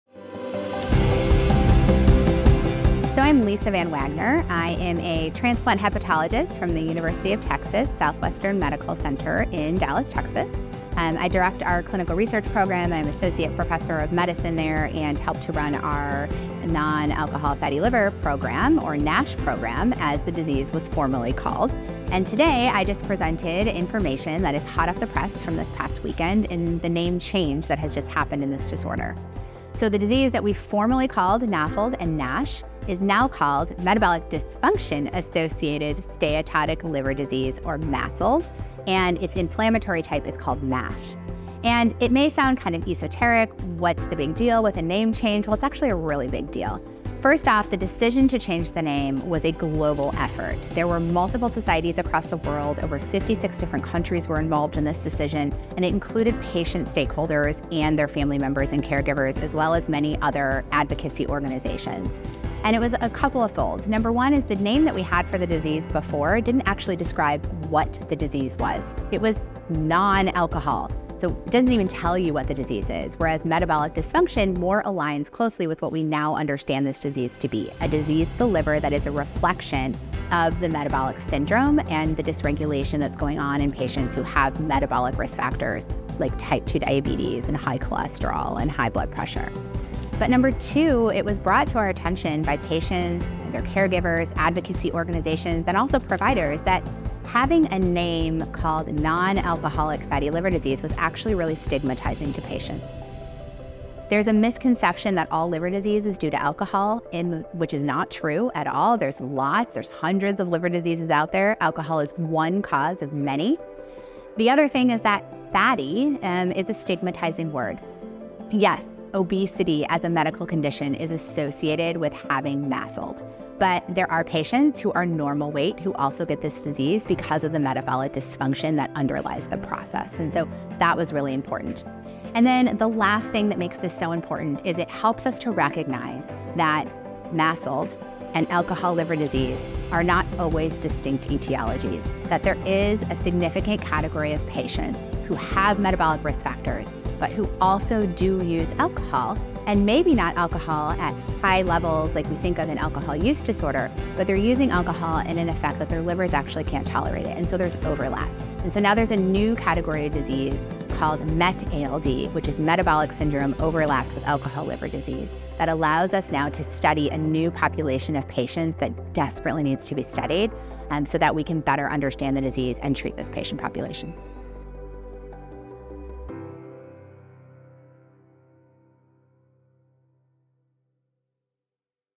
New Nomenclature - NAFLD is now Metabolic Dysfunction-Associated Steatotic Liver Disease (MASLD)
Join us for an exclusive interview with Dr. Lisa VanWagner, a distinguished hepatologist and Associate Professor of Medicine at the University of Texas Southwestern Medical Center. In this captivating video, Dr. VanWagner discusses the groundbreaking advancements in liver disease nomenclature, including the introduction of metabolic dysfunction-associated steatotic liver disease (MASLD) and metabolic dysfunction-associated steatohepatitis (MASH).

This enlightening conversation took place during Dr. VanWagner's presentation at the 31st Annual Primary Care Conference in Kiawah Island, SC. At this event, she shared her expertise with a diverse audience of primary care professionals, emphasizing the importance of accurate diagnosis and management of steatotic liver disease.

As you watch this interview, you'll gain valuable insights into the newly introduced MASLD and MASH terminologies and their significance in the field of hepatology. Dr. VanWagner's presentation at the Primary Care Conference provided primary care practitioners with a comprehensive understanding of these concepts, empowering them to better identify and address liver-related conditions in their patients.

Join us in spreading awareness and improving patient outcomes through evidence-based approaches and ongoing professional development.

Don't miss this exclusive interview with Dr. Lisa VanWagner, where she shares her expertise on MASLD, MASH, and other crucial aspects of liver diseases. Together, let's enhance our understanding and management of liver conditions, driving progress in the field of hepatology and positively impacting patient care.